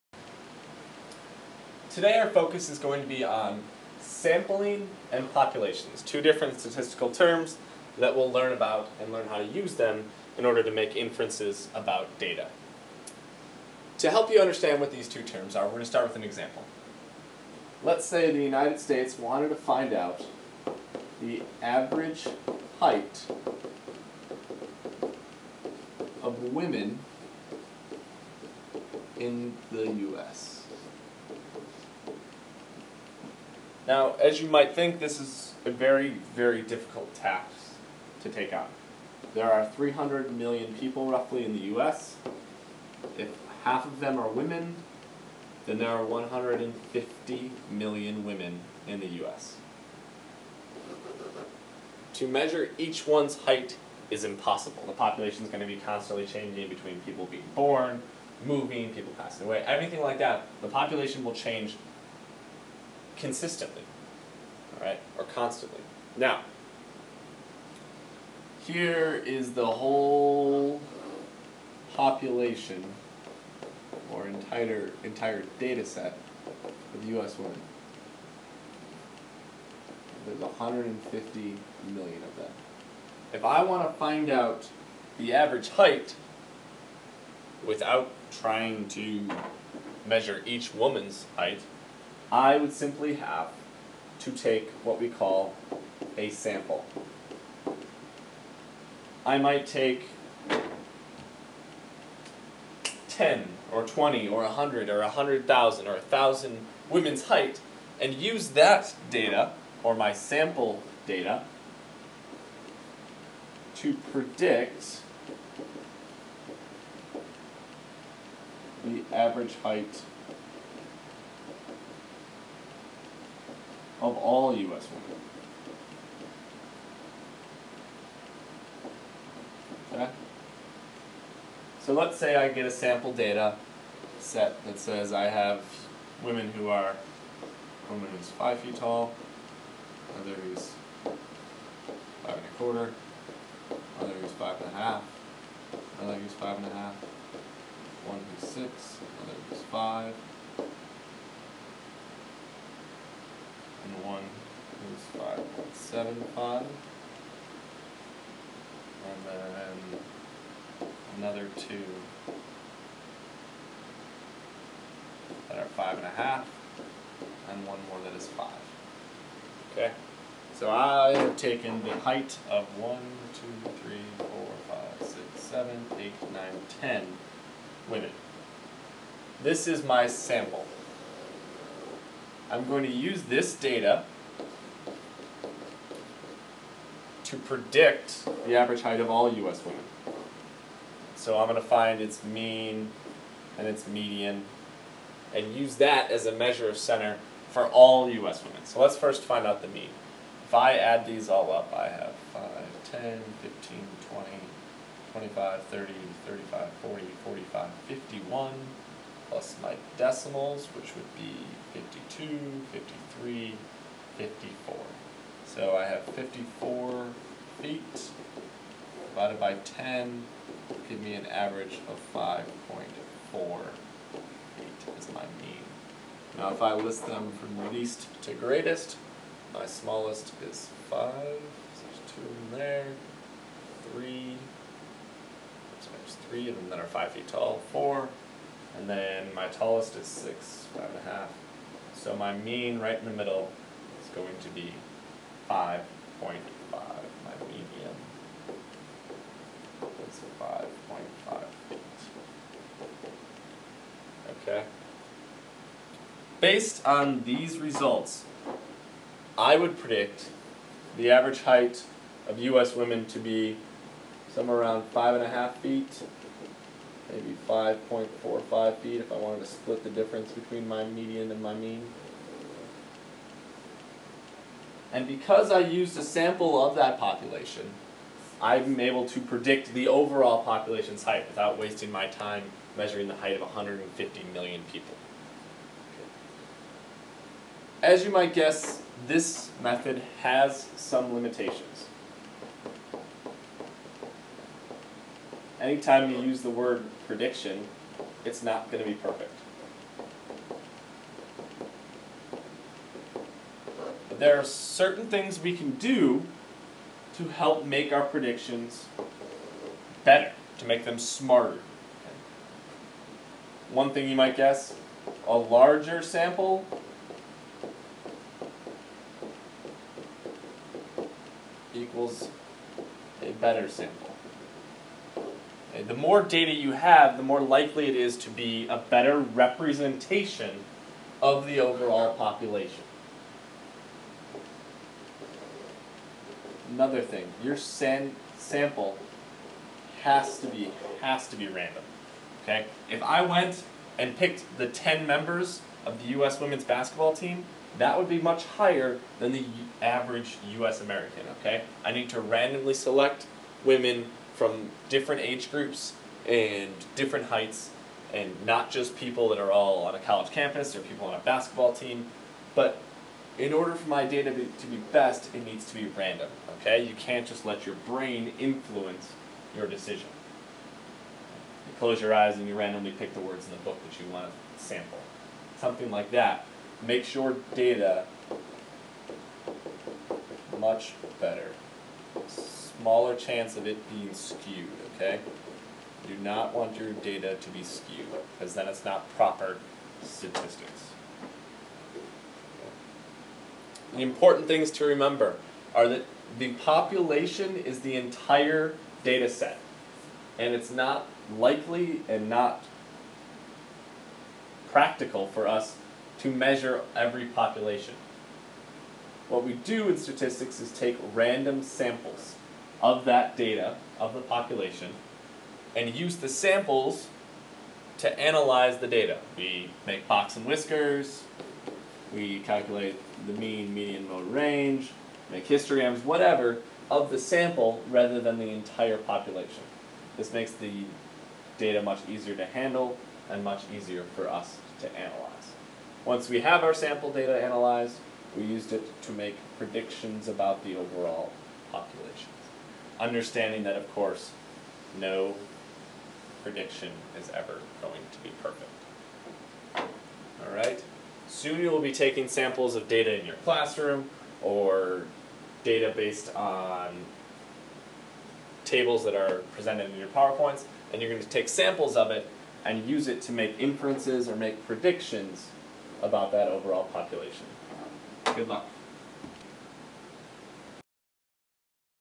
7th LP4 Day 1 Random Sampling Introduction
Random sampling and inferences made about populations.